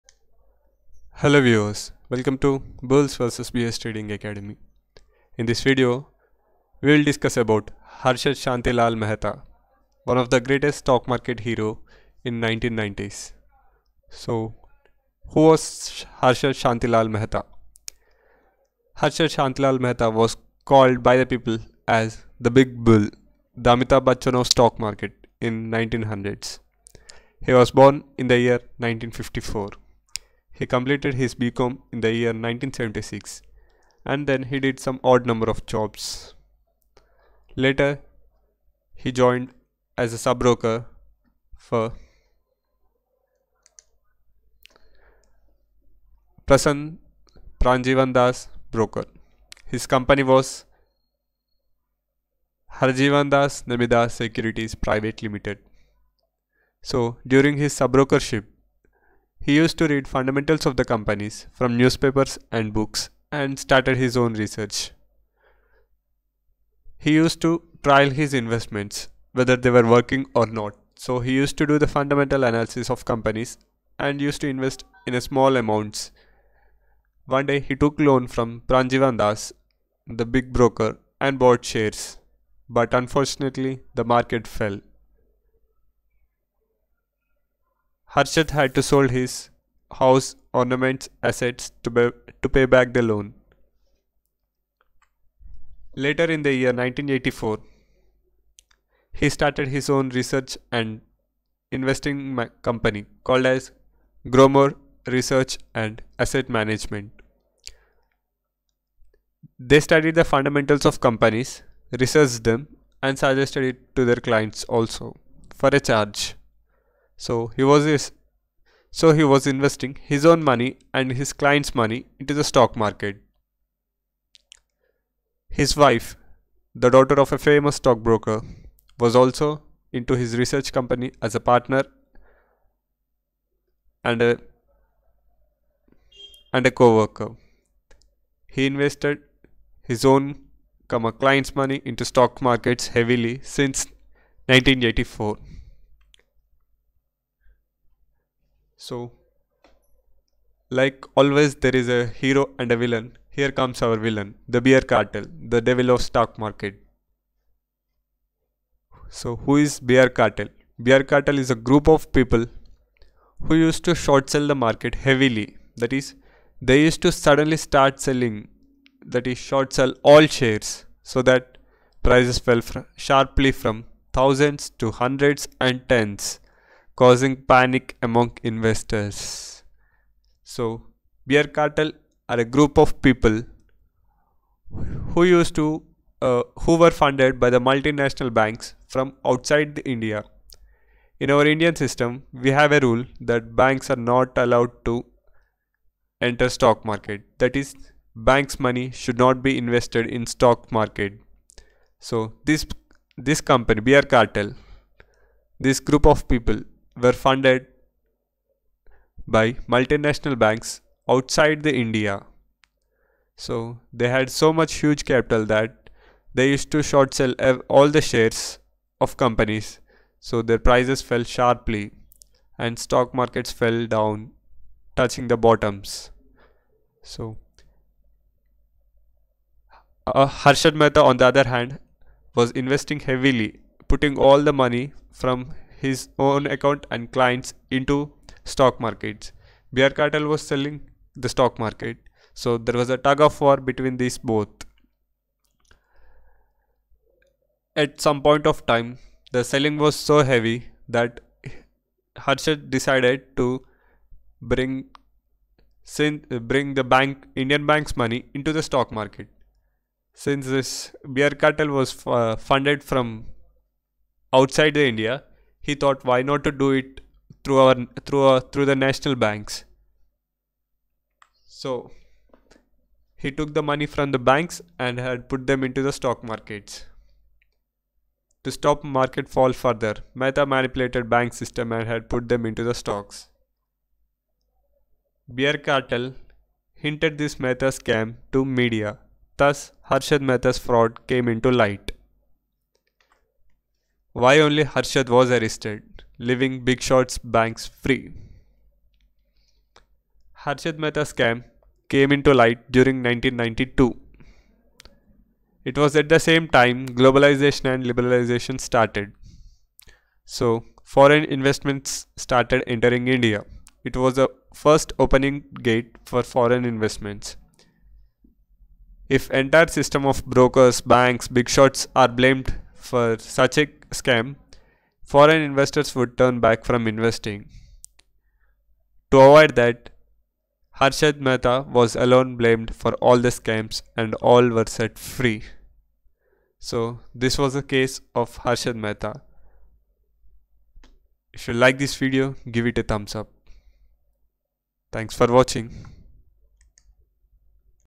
HARSHAD MEHTAS COMPLETE STORY AND SCAM.
THIS VIDEO EXPLAINS THE STORY OF HARSHAD MEHTA-THE BIG BULL OF INDIAN STOCK MARKETS AND HIS SCAM,HIS ENELY THE BEAR CARTEL.